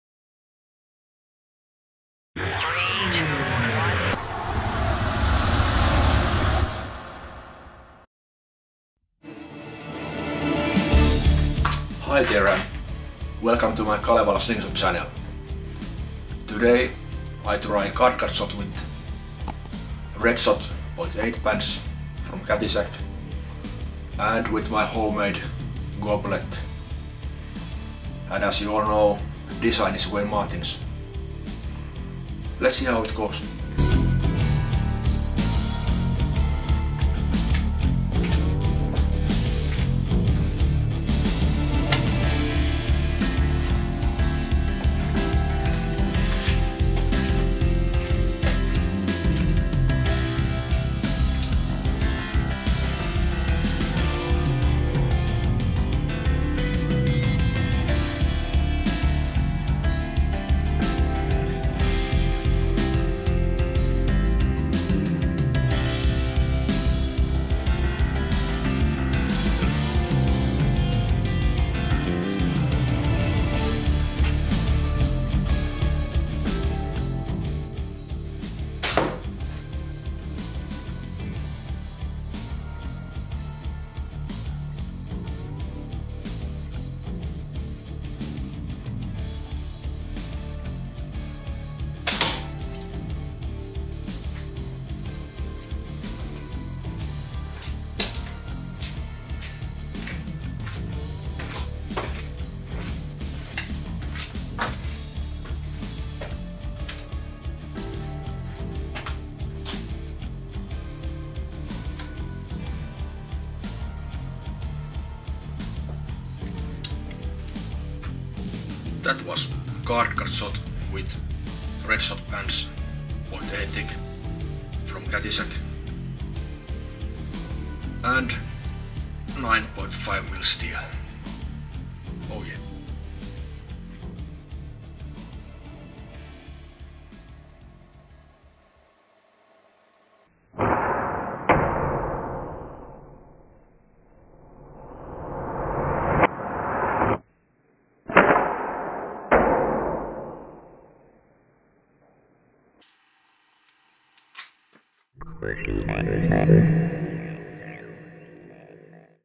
Red Shot 0,8 & Homemade Goblet & Card Cut Shot
05/11/2022

Been awhile since my last time shooting in the basement.
No wind and no rain. Nice 👍.
Also couple weeks since my last card cut.
...and also almost three weeks since my last time shooting with slingshot.